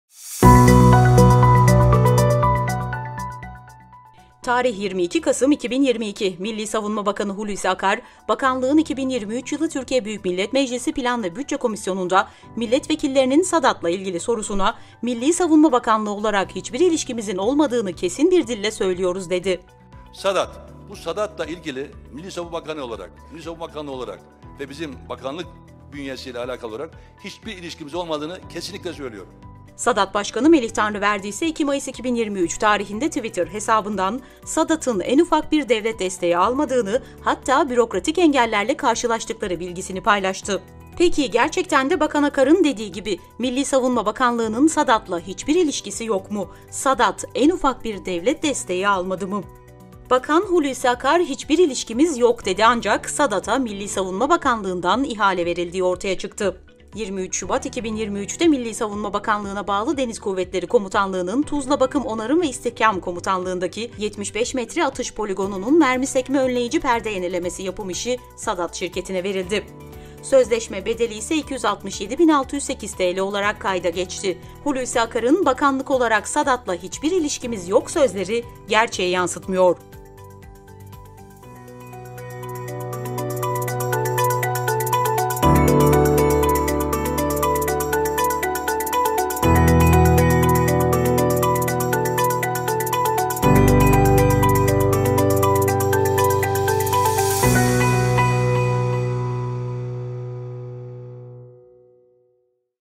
Milli Savunma Bakanlığı’nın SADAT ile hiçbir ilişkisi yok mu?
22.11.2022 - Hulusi Akar - Milli Savunma Bakanı
❌ “Bu SADAT ile ilgili Milli Savunma Bakanı olarak, Milli Savunma Bakanlığı olarak ve bizim bakanlık bünyesi ile alakalı olarak hiçbir ilişkimiz olmadığını kesinlikle söylüyorum.”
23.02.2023 – Milli Savunma Bakanlığı İhalesi
✅ SADAT’a Deniz Kuvvetleri Komutanlığı’na bağlı 267 bin 608 TL’lik bir ihale, MSB tarafından verildi